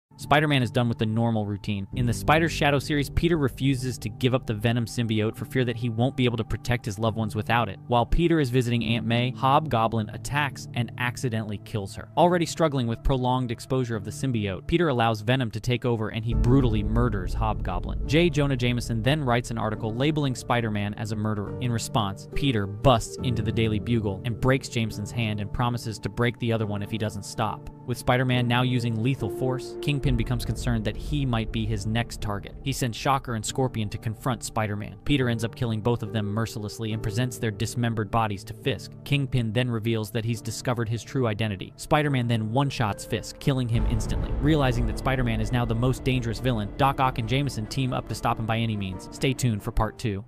Spider-Man STOPS Holding Back
Spider-Man: Spider's Shadow 1-2

Spider-Man is done with the normal routine. In The Spider's Shadow series, Peter refuses to give up the Venom symbiote for fear that he won't be able to protect his loved ones without it. While Peter is visiting Aunt May, Hobgoblin attacks and accidentally kills her. Already struggling with prolonged exposure of the symbiote, Peter allows Venom to take over and he brutally murders Hobgoblin. J. Jonah Jameson then writes an article labeling Spider-Man as a murderer.

In response, Peter busts into the Daily Bugle and breaks Jameson's hand and promises to break the other one if he doesn’t stop. With Spider-Man now using lethal force, Kingpin becomes concerned that he might be his next target. He sends Shocker and Scorpion to confront Spider-Man. Peter ends up killing both of them mercilessly and presents their dismembered bodies to Fisk. Kingpin then reveals that he’s discovered his true identity. Spiderman then one-shots Fisk, killing him instantly. Realizing that Spider-Man is now the most dangerous villain, Doc Ock and Jameson team up to stop him by any means.